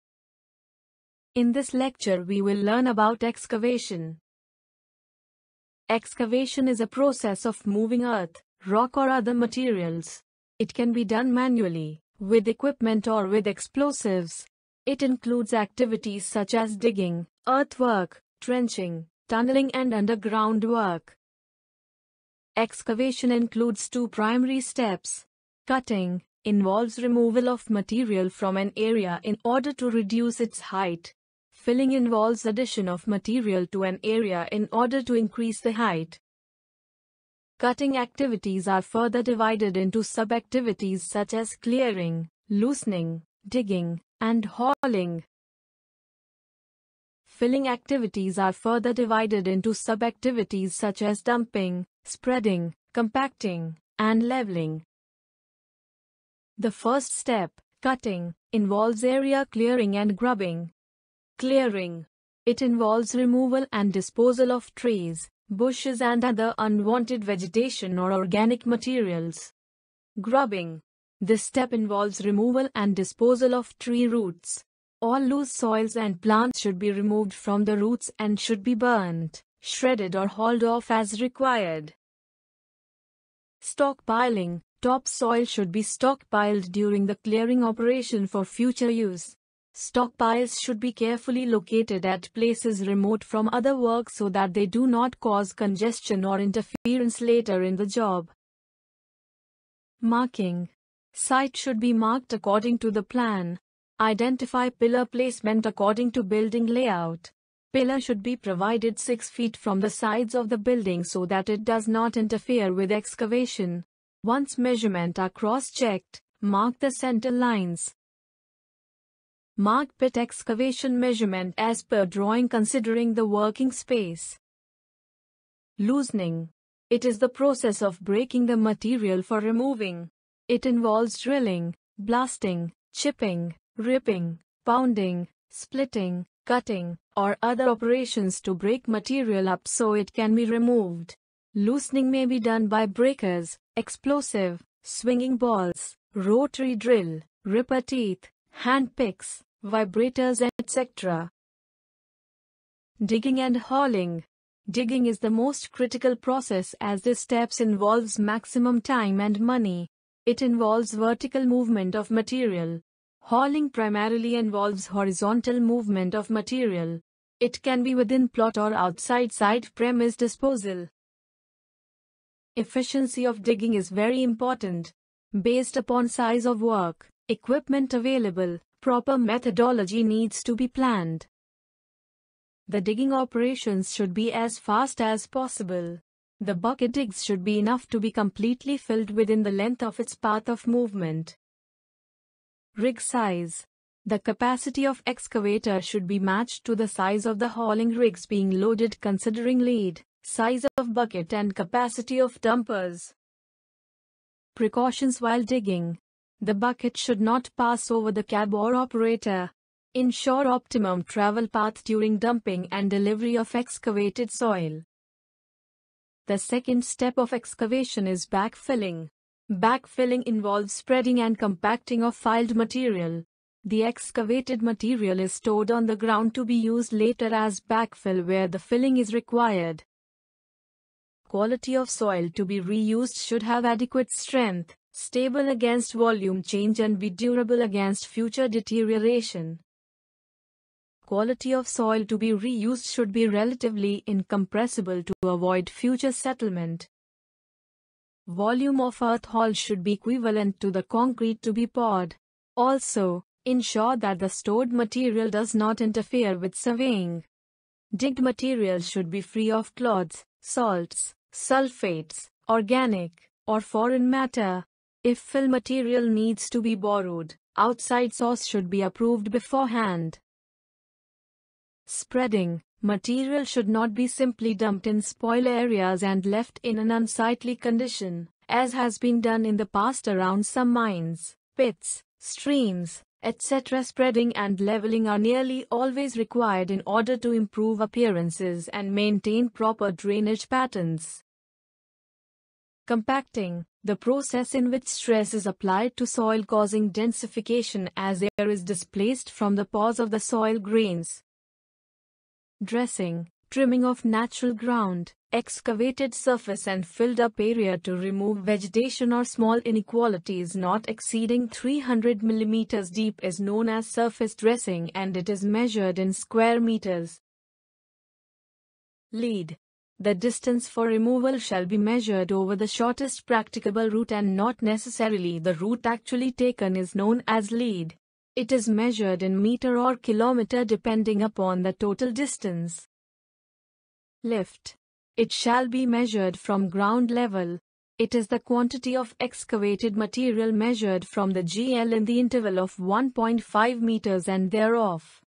1 01 N Basics of Excavation
In this lecture, we will learn about the steps involved in Excavation, precautions while conducting excavation etc. and the different terms like Surface dressing, Lead, Lift etc.